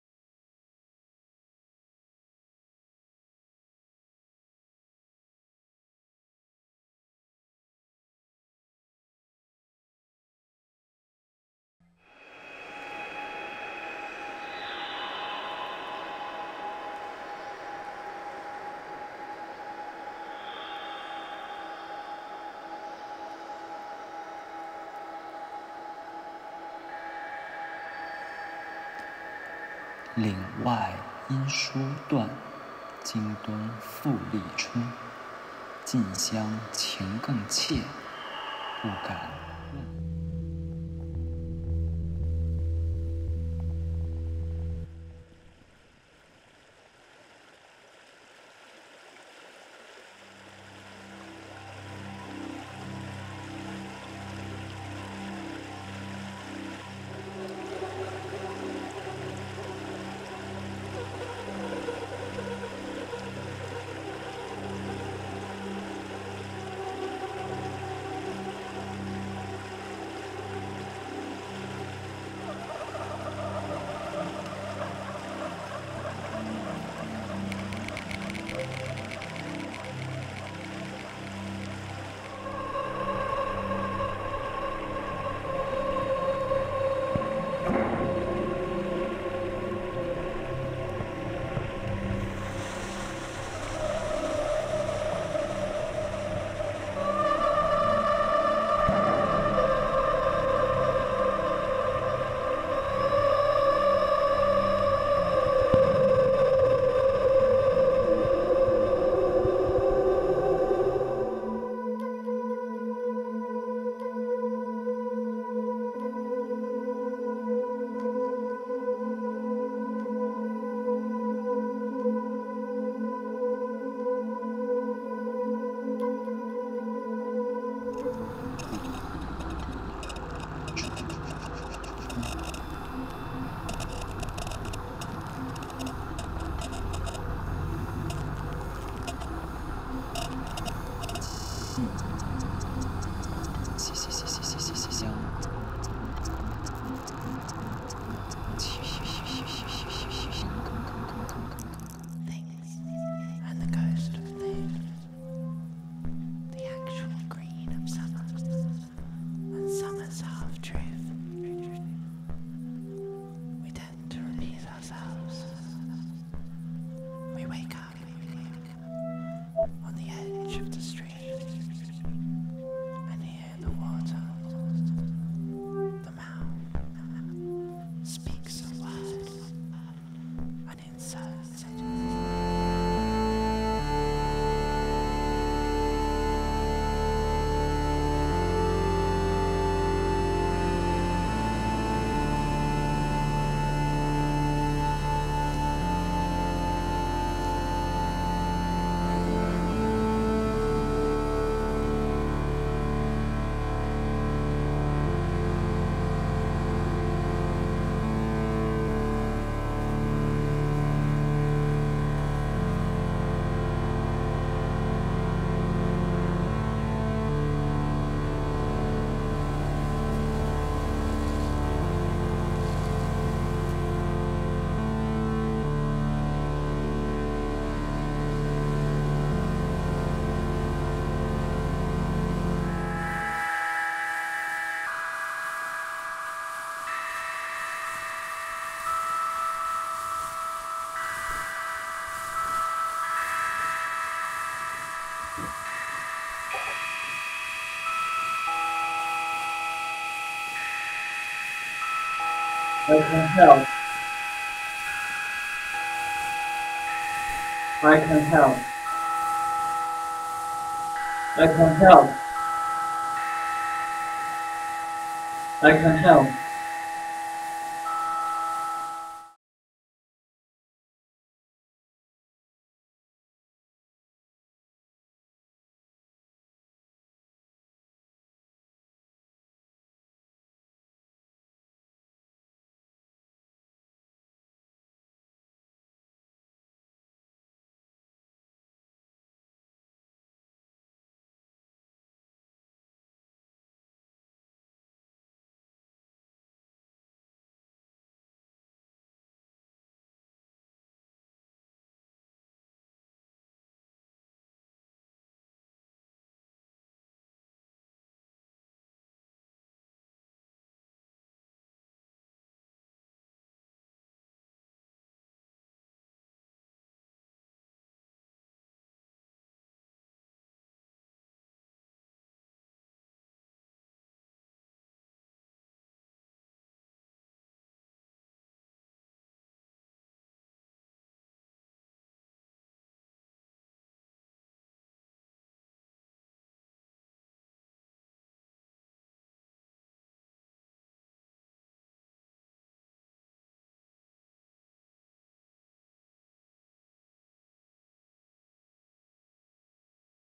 The river of no one, DAP-Lab
Their final 30-minute performance, The river of no one was performed in six parts and included multiple video and photographic images, scene transitions and audio accompaniments. This was a fluid performance that journeys through place and time, literally and figuratively, with a uniquely personal, yet collective voice. Each scene utilised unique virtual sets made up of the composited video and photographic imagery, including a recorded video performance by Johannes Birringer. The two live performers were placed in various quadrants of the Telepresence Stage environment, and often scaled to be small or large. At times, special effects were applied such as different transparency/opacity ratios to create a ghostly ethereal presence. Much was made of a rich layering of visual content, for example, with images of watercolour paintings surrounding a performer, who appears to move within a painting in progress.